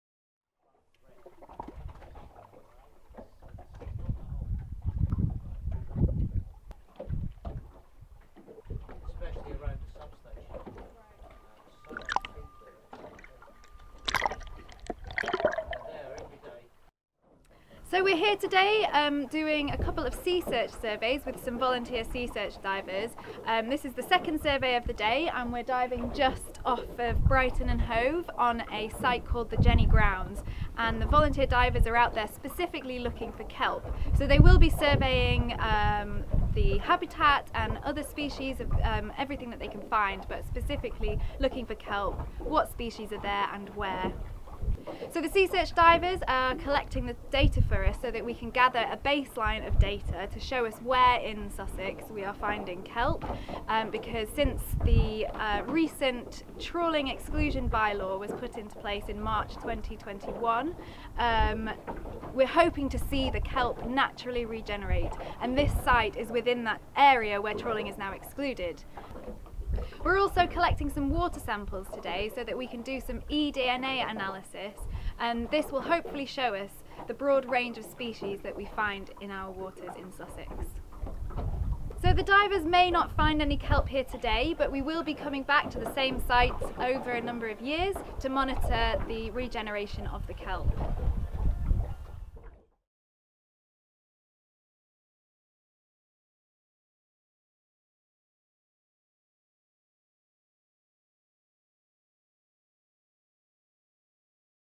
Seasearch and Sussex Kelp Restoration Project
Using cutting edge science and technology the Sussex Kelp Restoration Project research is collecting environmental DNA for analysis to create a biodiversity baseline, assisted by the Seasearch divers.